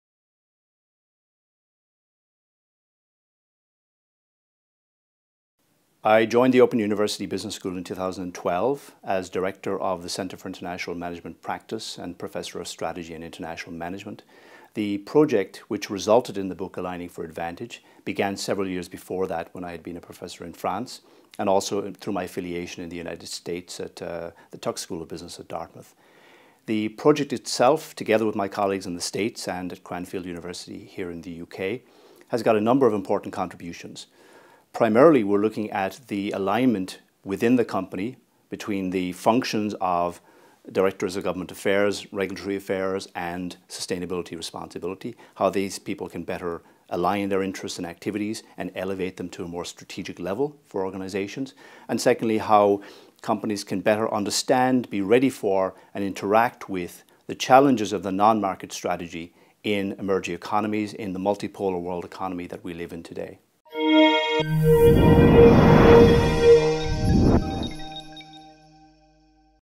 Aligning for Advantage' by Professor Thomas C. Lawton
Professor Thomas C. Lawson, Professor of Strategy and International Management at The Open University Business School, highlights the key contributions of his latest book. In Aligning for Advantage, Professor Lawton and the co-authors argue that to successfully deliver a company's overarching purpose and intent, competitive strategy needs to be synchronized with strategies for political and regulatory activism and social and environmental engagement.